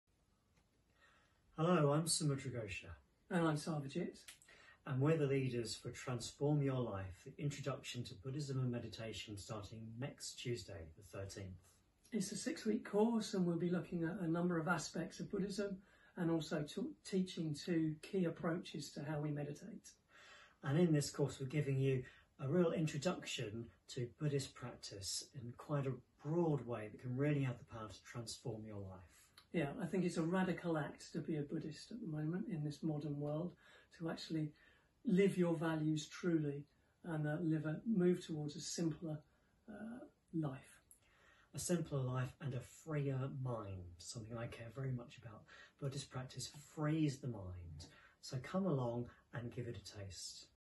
Transform Your Life course starting 13 Sept
Samudraghosha and Sarvajit invite you to join their course